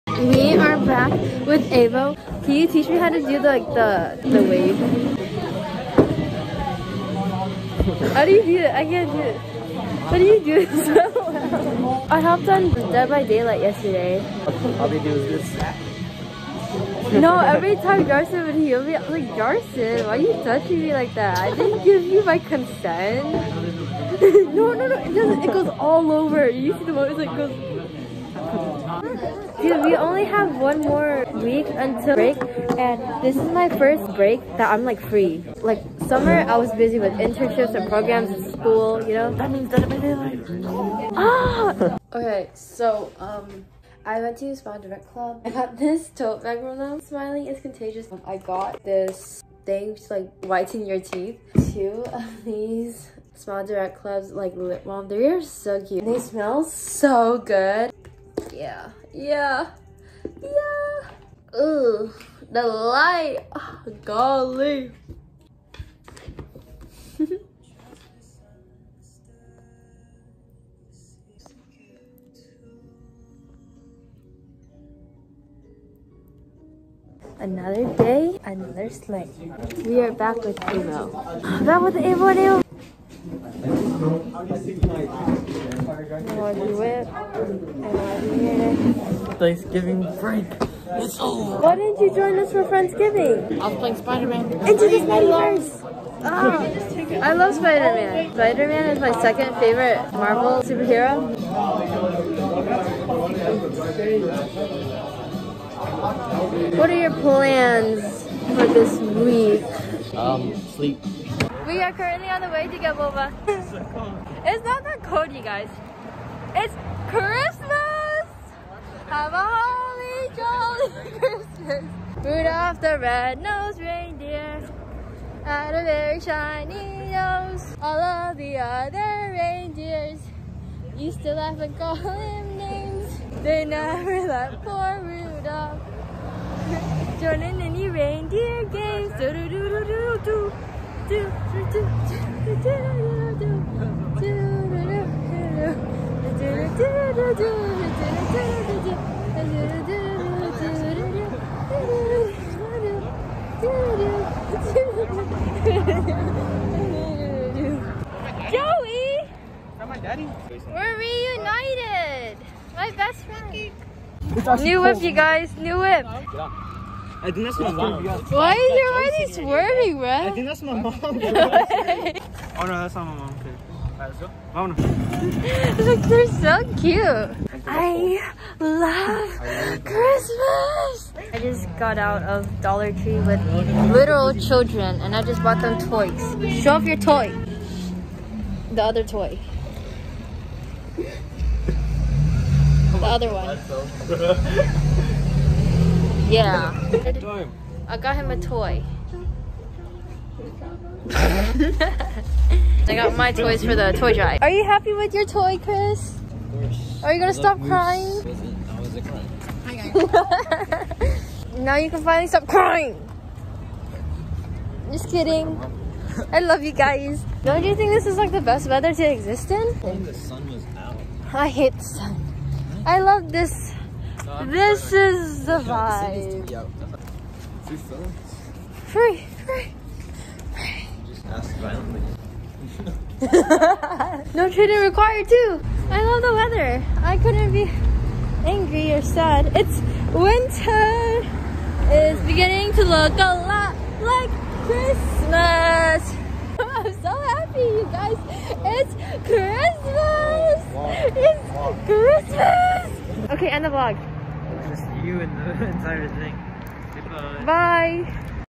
dec 1: I LOVE CHRISTMAS!! | week 13
hi friends, i don't usually write anything in the description but this was a weird week that consisted of deadbydaylight talk w/ avo, christmas toy drive prep, chris crying at tisane, PERFECT HOLLY JOLLY SEASON